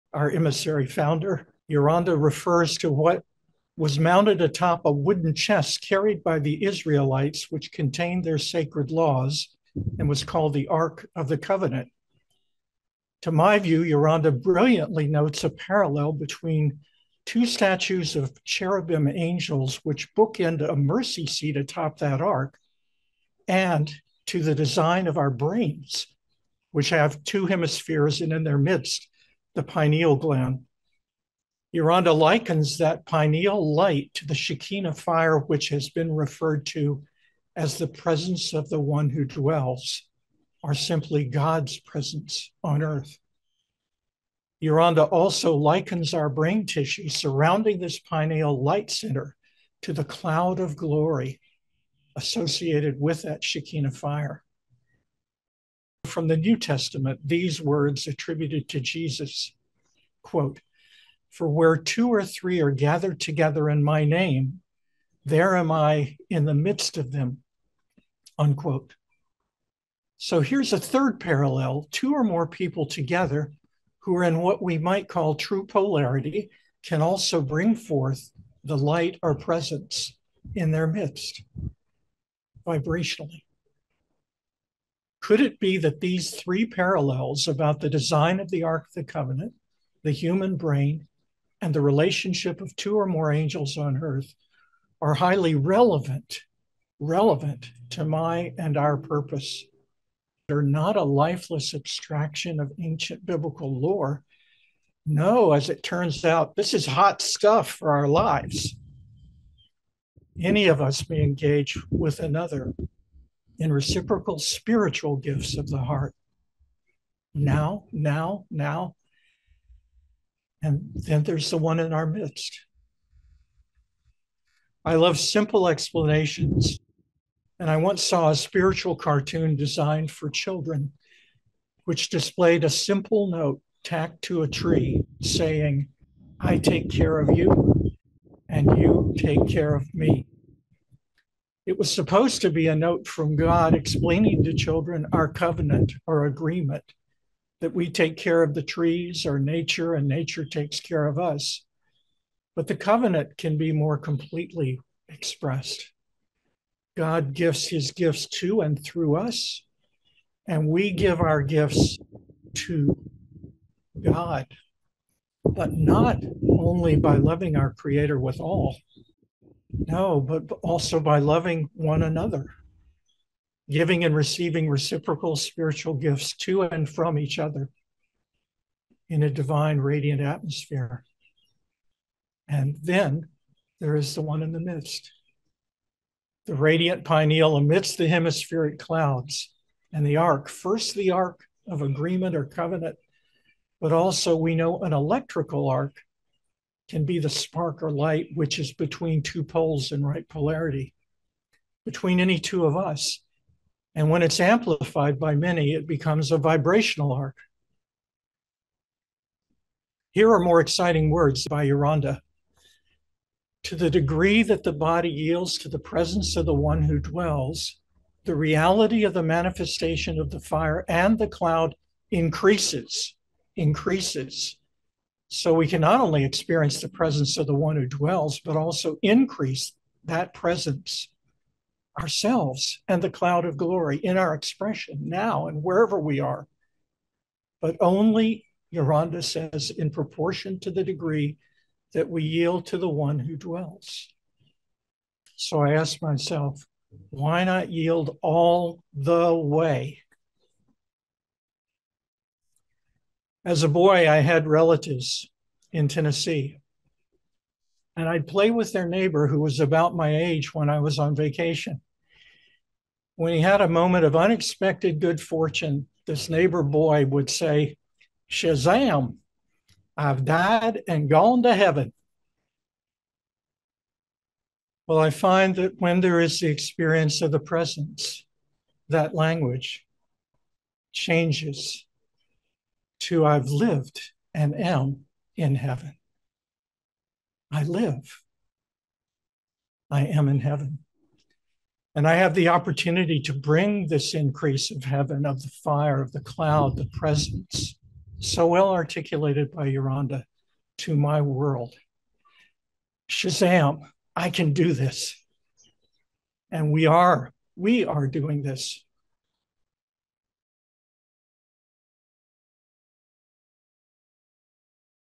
Tom Cooper: Why Not Yield All The Way
In this enlightening excerpt from our Sunday Attunement Service at Sunrise Ranch in Loveland, Colorado on March 3rd, 2024, longtime Emissary Tom Cooper explores profound parallels between the design of the Ark of the Covenant, the human brain, and spiritual presence in "Why Not Yield All The Way".

Through engaging discourse and personal anecdotes, Tom delves into the transformative potential that arises when individuals align in true polarity, suggesting that they can manifest divine presence. Drawing on biblical references and spiritual insights, he discusses how complete yielding to divine influence can amplify spiritual presence, emphasizing the reciprocal exchange of spiritual gifts between individuals.

Join us in this transformative discussion as we explore the depths of spiritual presence and alignment. Don't miss out on these valuable insights!